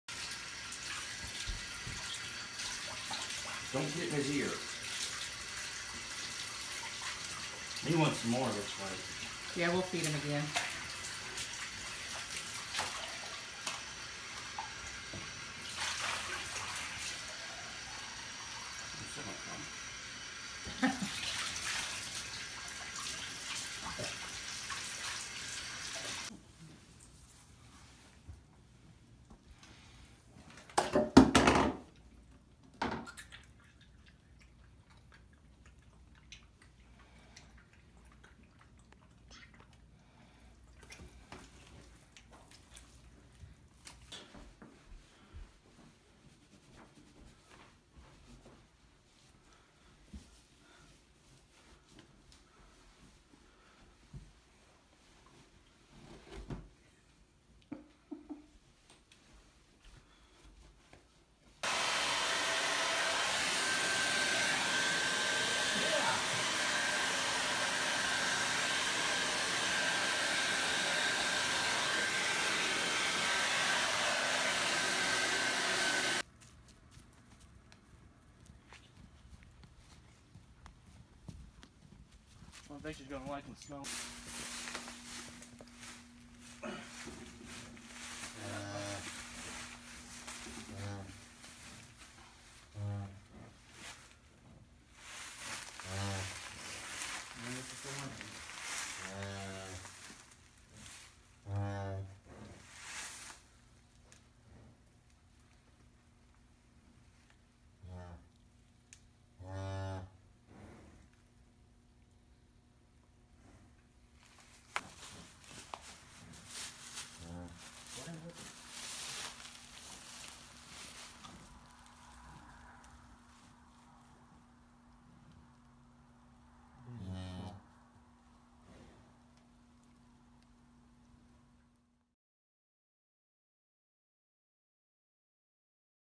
Cow in the house in the bathroom in the tub
A heifer had a bull calf in the mud. So we brought the baby cow in the house into the bathroom. In the tub we cleaned him up. We also fed him colostrum to get him going. Once he was dried off, the baby bull was carried out to the cattle shed. Then placed back with his mother in a calving pen. He will be "right as rain" now.